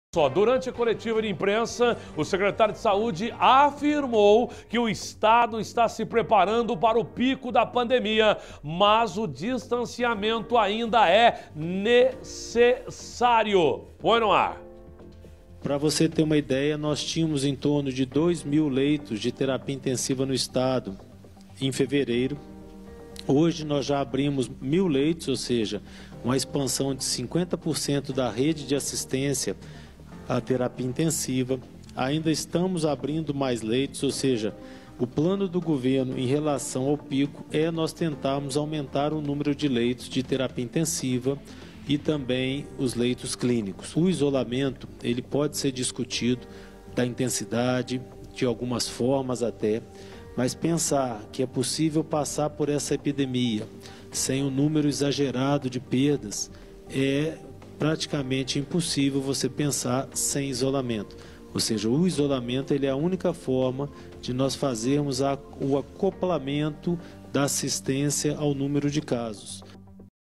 MG: Pico da pandemia
MG: SECRETÁRIO DE SAÚDE FALA DO PICO DA PANDEMIA | Segundo o Governo do Estado, MG está preprada para o pico que deve acontecer em 19 de Julho.